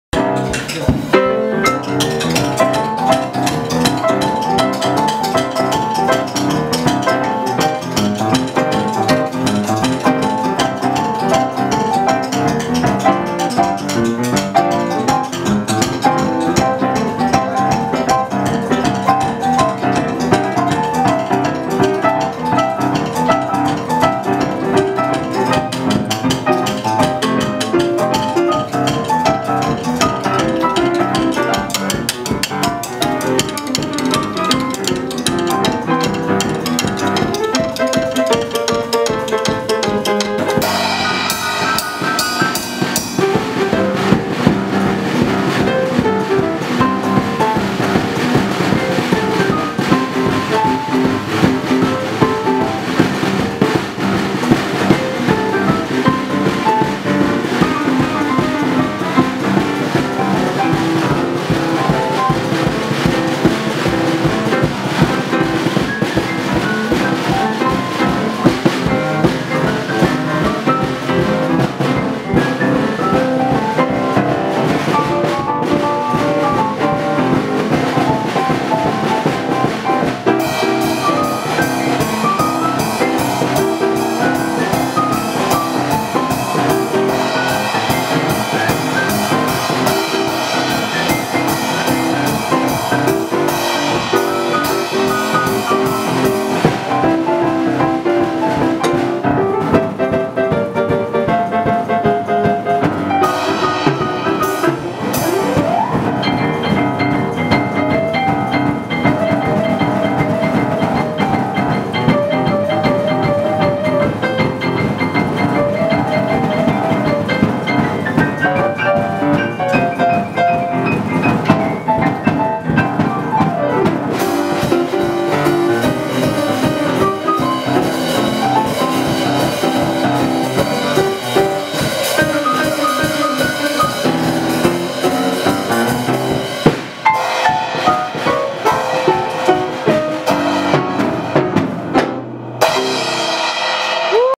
World's oldest living "Boogie Woogie" drummer
Practicing for upcoming big bash. Leo hadn’t played in public for 25 years... so he is
determined to learn his part well. He got the job!
Oldest son David playing Boogie Woogie with his wife Karen. Dad is now (11/23) 103 years and one month old!!